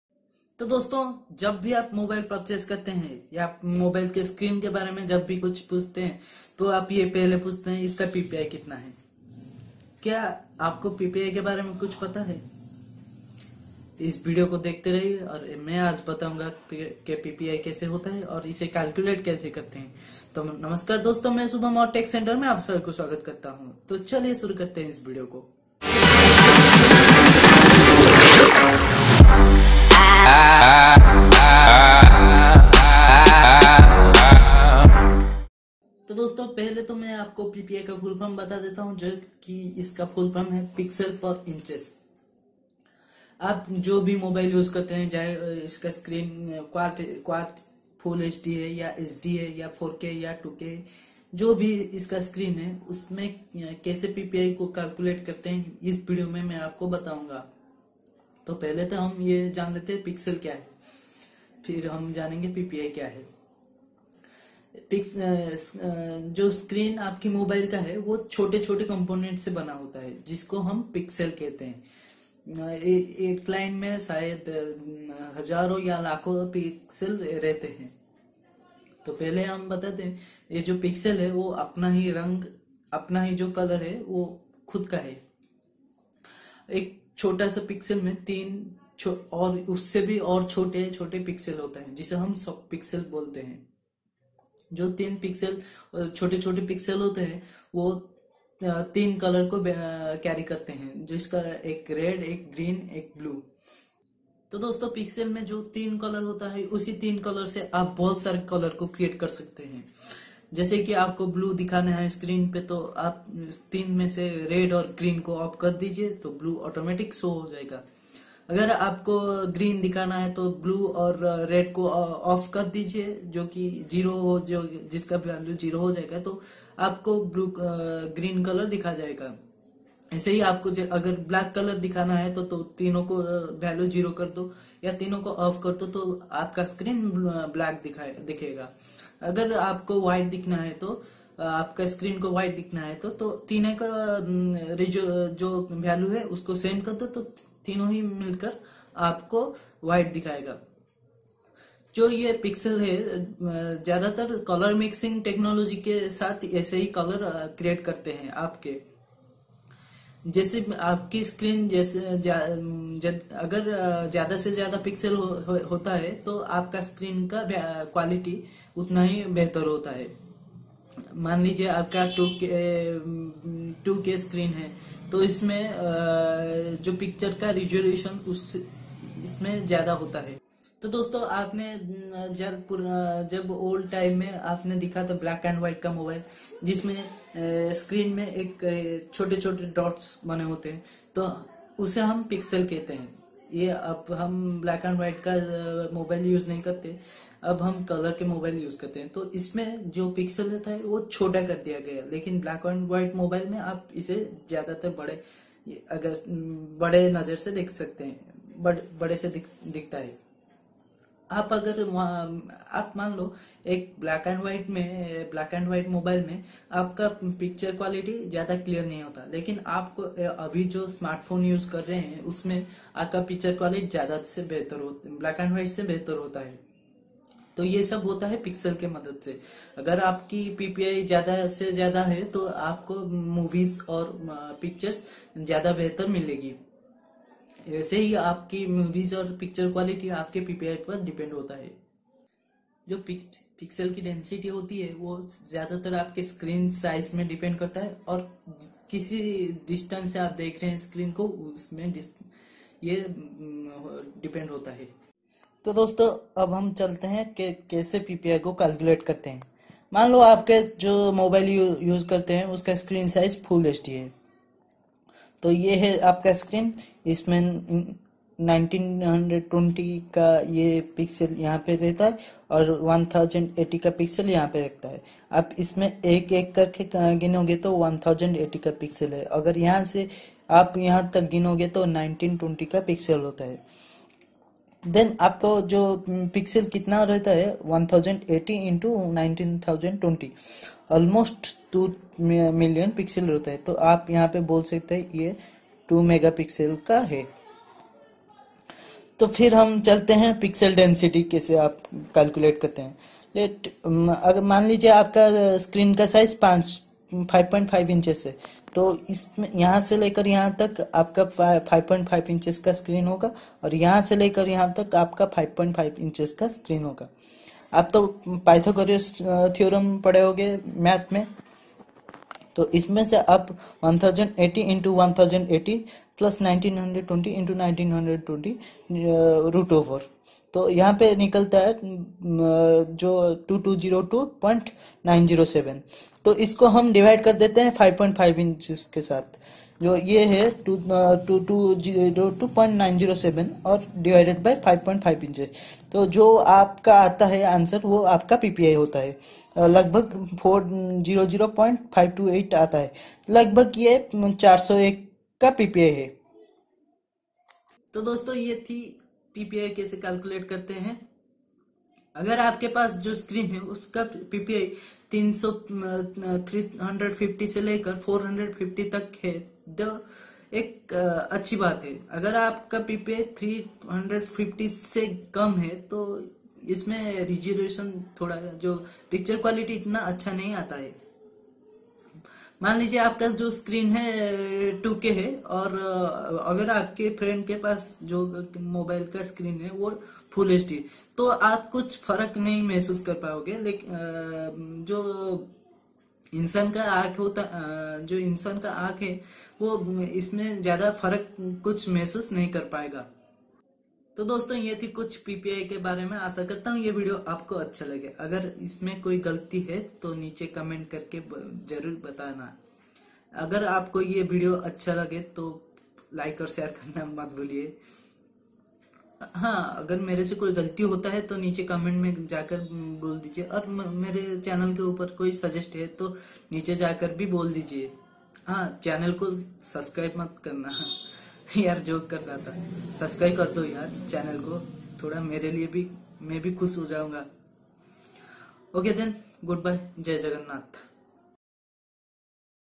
Explained About PPI !!!!!!! How toCalculate the PPI ????????????
Namaskar Dosto is Video main Aaj Screen ki PPI ke bare main baat kiya hun . aur PPI ko kaise Calculate karte hain Wo bhi Bataya . To dosto Ye video kaisa laga aap plz niche comment karke bataye. Agar mere se koi galti hui he aur mere channel ke upar koi suggest hai to jaroor bataeyein . Ok then Good Bye. Jai Jagganath .